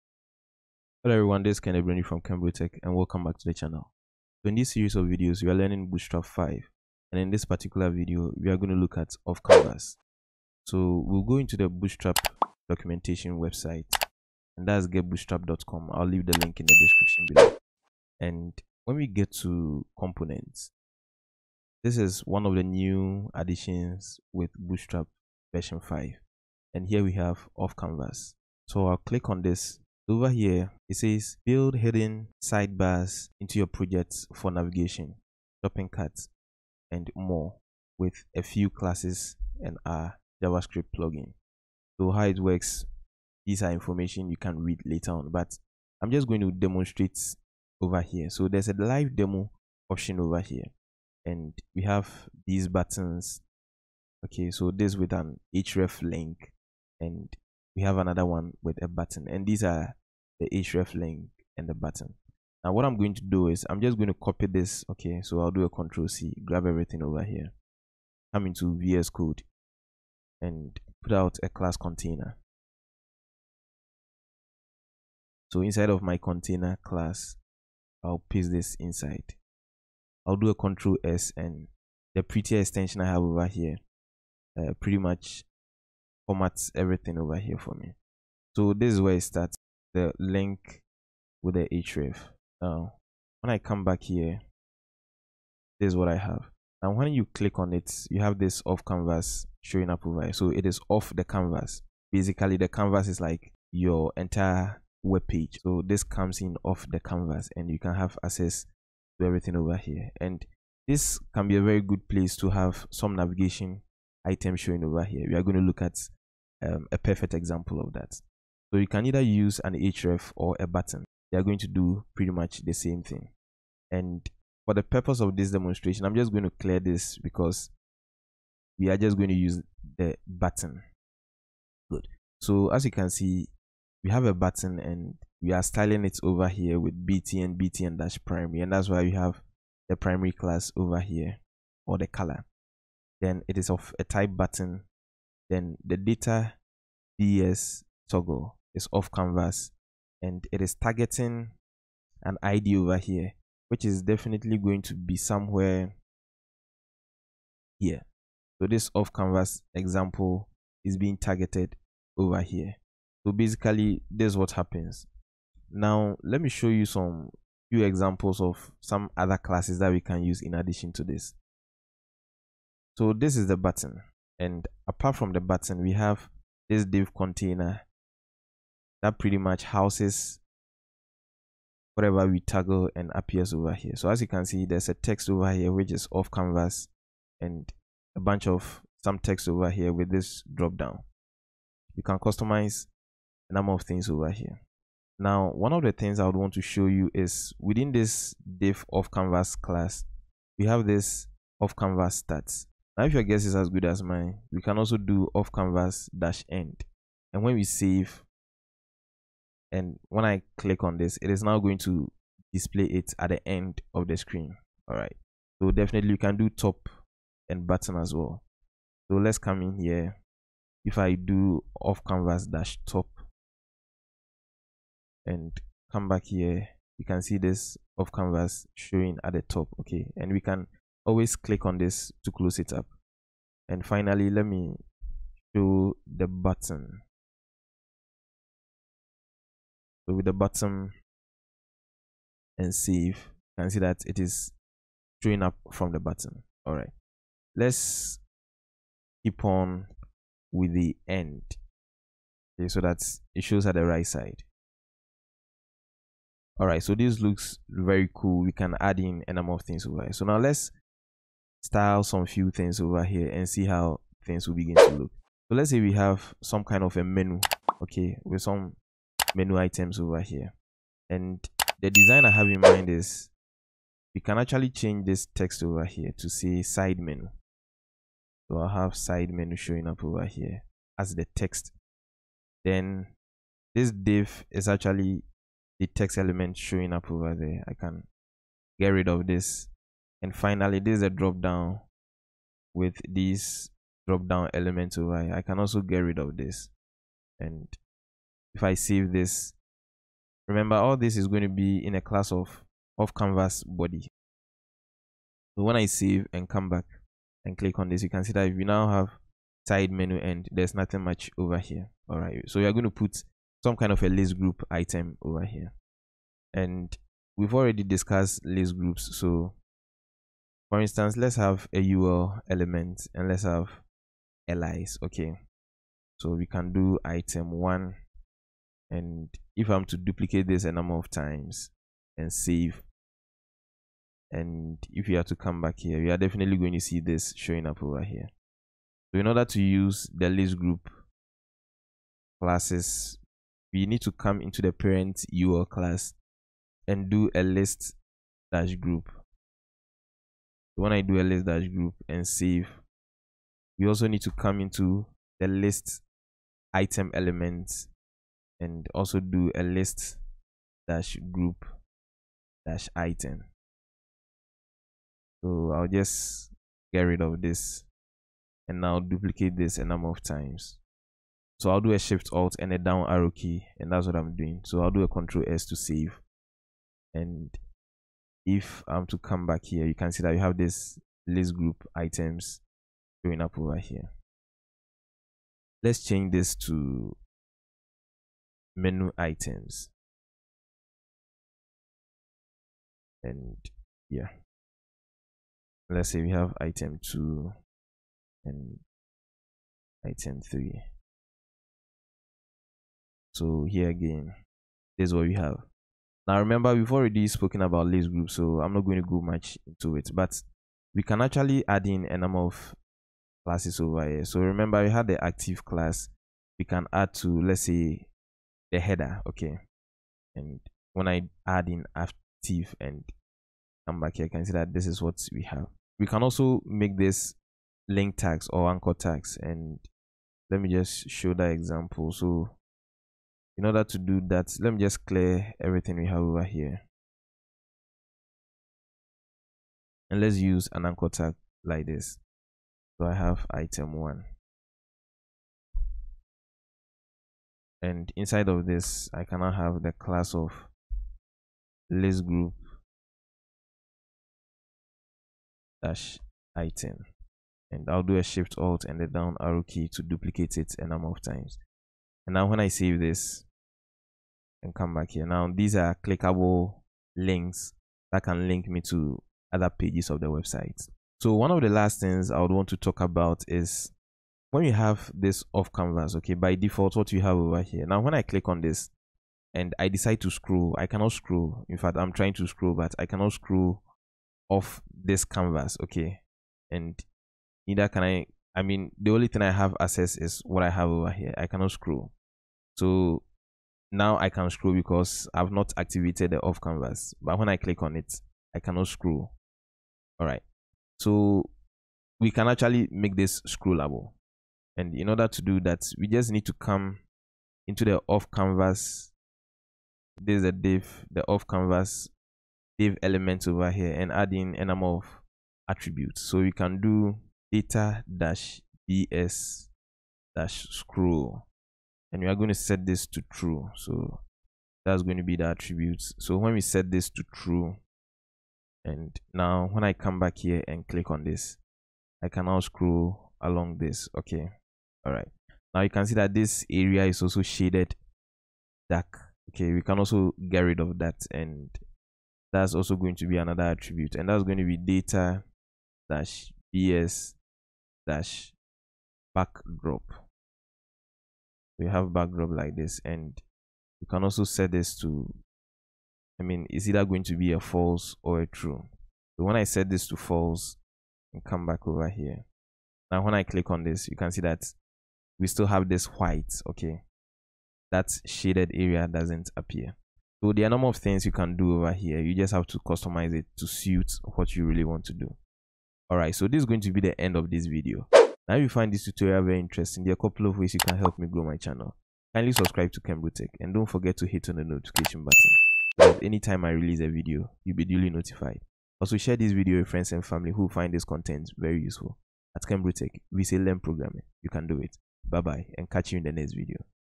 Bootstrap 5 Crash Course Tutorial #14 - Offcanvas
In this series of videos, we will learn Bootstrap 5. In this video you will learn how to make use of the Bootstrap 5 Offcanvas.